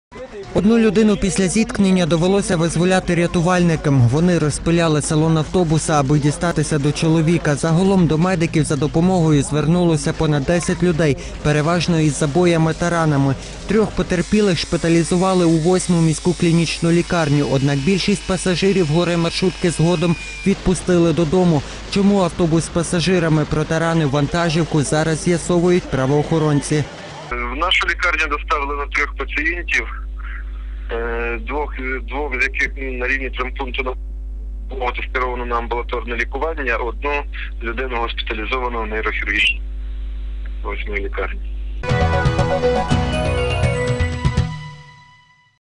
ДТП Львів-Пустомити
Вісім дорослих та троє дітей. Стільки людей постраждали підчас аварії маршрутки Львів  - Пустомити. Надзвичайна подія трапилася в суботу поблизу Львова. Наповнена людьми маршрутка врізалася в припарковану на узбіччі вантажівку.